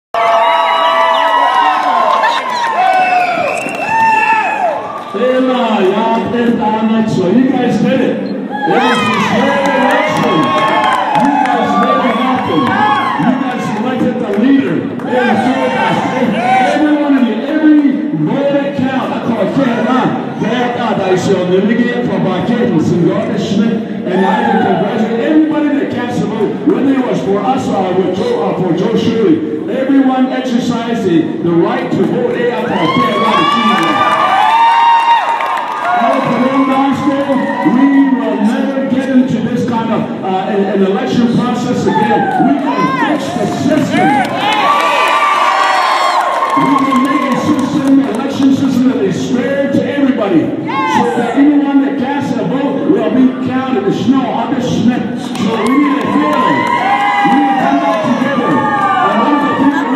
President-elect Russell Begaye speaks to the people gathered inside the sports center
By: Noel Smith - Reports about the Navajo Nation for The Daily Times in Farmington, N.M.